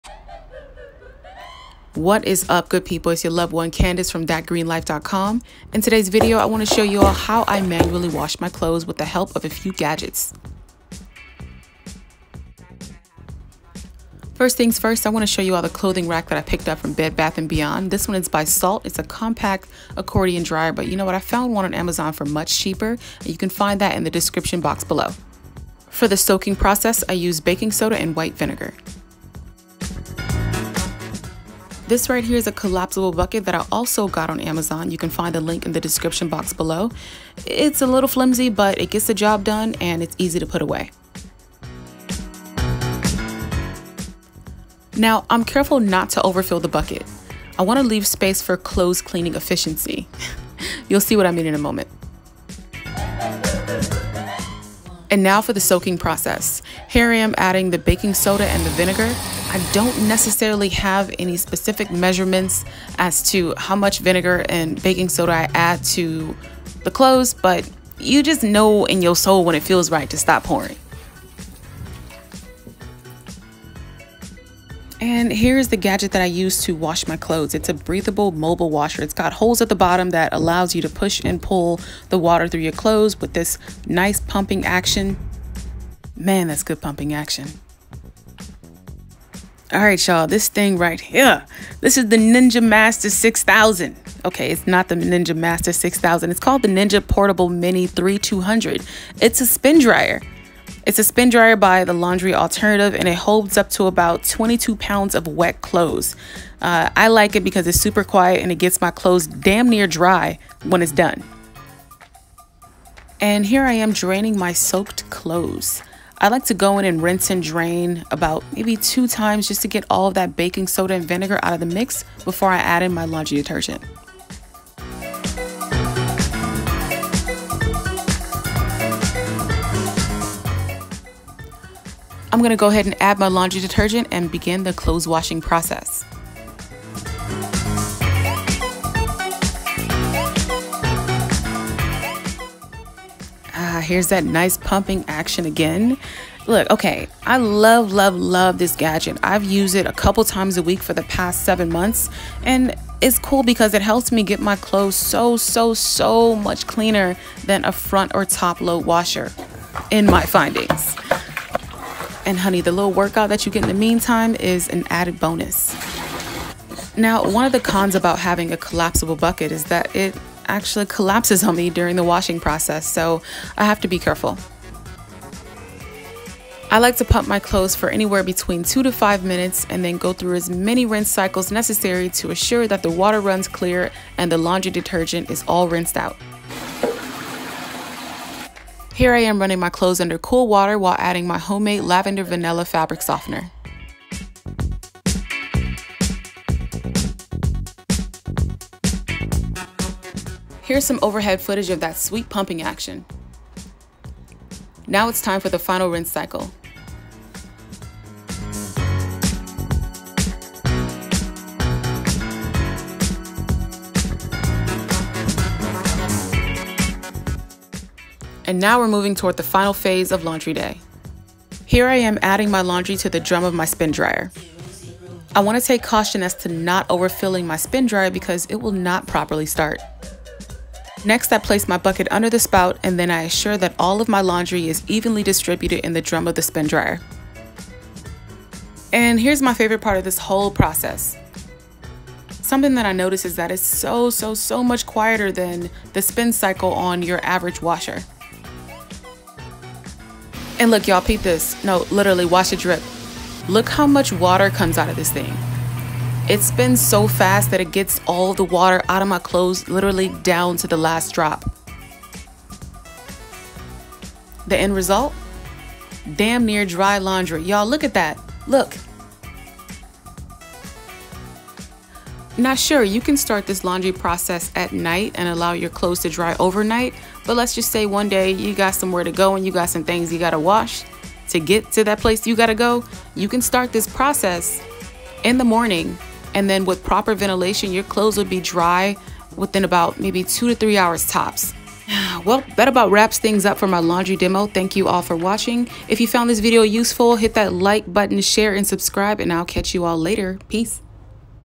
How to Do Laundry Without a Washing Machine
In this video, I give you all a peek into my Laundry Day. I've been washing machine free for a little over 2 years now and I absolutely love it!

While eco-friendly, this method is NOT for everyone. I choose to wash my clothes this way because as an active fitness professional who can go through about 3 changes of clothes in a day, laundry adds up...QUICK! It saves me $$, time, and a trip to the laundry mat. It’s also convenient because I don’t like a full hamper of dirty clothes just waiting for me to take them to the laundry mat only to get 65% clean and half-way rinsed. Washing my clothes this in this manner is a sure way for me to get my clothes clean, rinsed thoroughly and smelling fresh—just the way I like it!
See blog post:
Caveat:
 I do in fact take my bath towels and blankets to the laundry mat because they would surpass the "wet laundry weight limit" of 22 lbs. I do NOT fill the washer drum near the recommended “fill line”. It’s the best way to get my towels and blankets more properly cleaned and rinsed.
Tools I used:
Breathable Washer
Ninja Spin Dryer
Collapsible Bucket
Accordion Clothes Drying Rack
White Vinegar
Baking Soda
My favorite Laundry Detergent (not used in video)